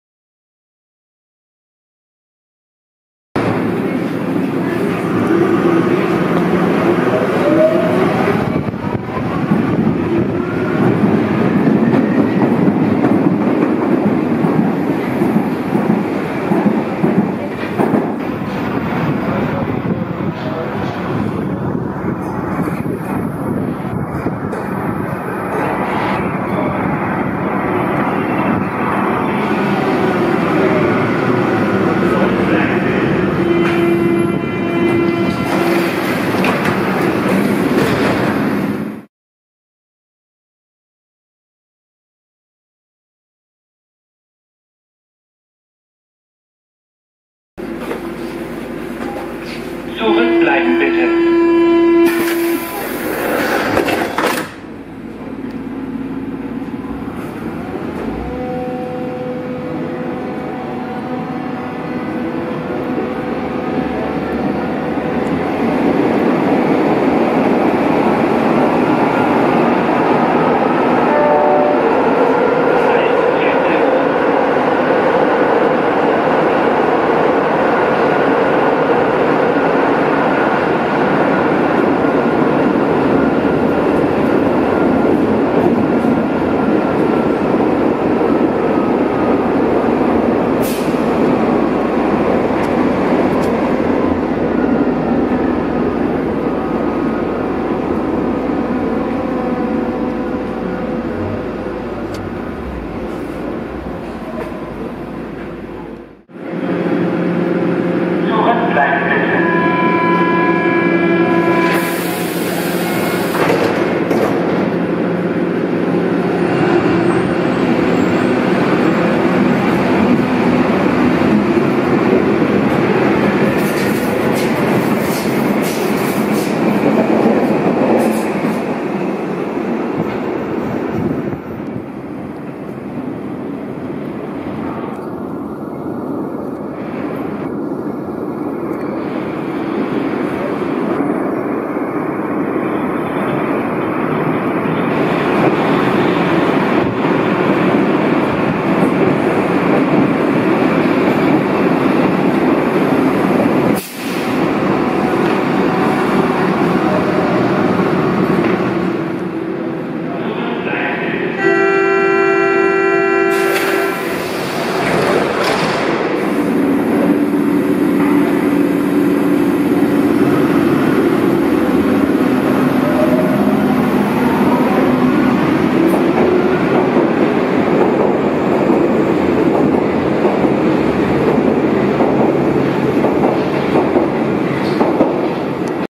U-Bahn Berlin: Das Unikat der Einheit 2826/2827 [1080p50-HD]
Hier geht es um die Einheit 2826/2827. Die ist die allerletzte Braune Einheit der BVG. Alle F84/F87 besitzen einen Grauen Innenraum aber das Holzinnenraum gefällt mir besser.